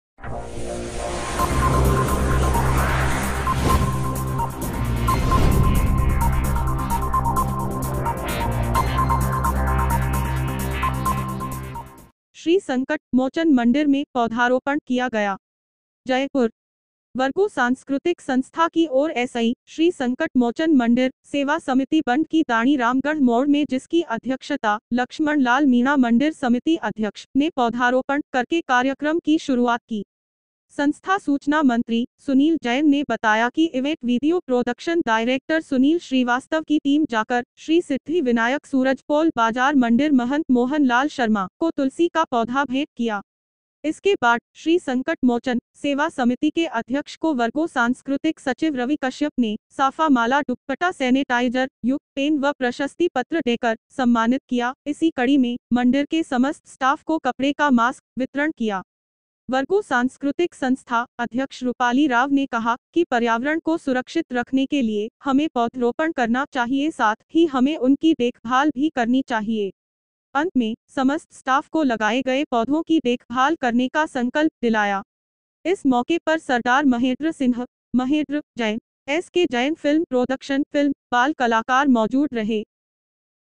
श्री संकट मोचन मंदिर में पौधारोपण किया गया
जयपुर । वर्गो सांस्कृतिक संस्था की ओर से श्री संकट मोचन मंदिर सेवा समिति बंद की ढाणी रामगढ़ मोड़ में जिसकी अध्यक्षता लक्ष्मण लाल मीणा मंदिर समिति अध्यक्ष ने पौधारोपण करके कार्यक्रम की शुरुआत की । संस्था सूचना मंत्री सुनील जैन ने बताया कि इवेंट वीडियो प्रोडक्शन डायरेक्टर सुनील श्रीवास्तव की टीम जाकर श्री सिद्धि विनायक सूरजपोल बाजार मंदिर महंत मोहन लाल शर्मा को तुलसी का पौधा भेंट किया । इसके बाद श्री संकट मोचन सेवा समिति के अध्यक्ष को  वर्गो सांस्कृतिक सचिव रवि कश्यप ने साफा माला दुप्पटा सेनेटाइजर युक्त पेन व प्रशस्ति पत्र देकर सम्मानित किया इसी कड़ी में मंदिर के समस्त स्टाफ को कपड़े का मास्क वितरण किया । वर्गो सांस्कृतिक संस्था अध्यक्ष रुपाली राव ने कहा कि पर्यावरण को सुरक्षित रखने के लिए हमे पौधरोपण करना चाहिए साथ ही हमे उनकी देखभाल भी करनी चाहिए । अंत मे समस्त स्टाफ को लगाए गए पौधों की देखभाल करने का संकल्प दिलाया । इस मौके पर सरदार महेंद्र सिंह, महेंद्र जैन, एस के जैन फ़िल्म प्रोडक्शन फ़िल्म बाल कलाकार मौजूद रहे ।